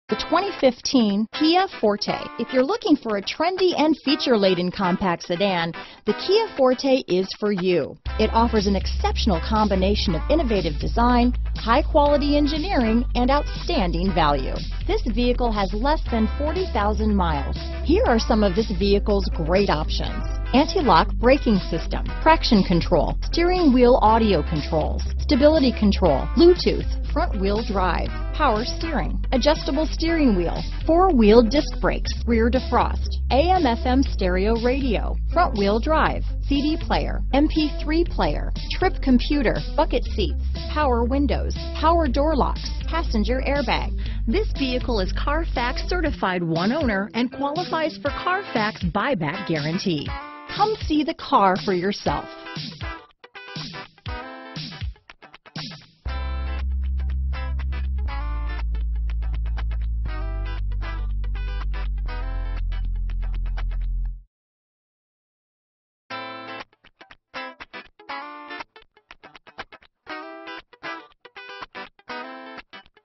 2015 Kia Forte Smyrna, Marrietta, Atlanta, Alpharetta, Kennesaw, GA 331069A
Used 2015 Kia Forte
Ed Voyles Hyundai Kia: Servicing Smyrna, Georgia near Marrietta, Atlanta, Alpharetta, and Kennesaw, GA.
2015 Kia Forte LX - Stock#: 331069A - VIN#: KNAFK4A60F5432658

2015 Kia Forte LX in Silver. New Price! CARFAX One-Owner.39/26 Highway/City MPG Odometer is 6077 miles below market average!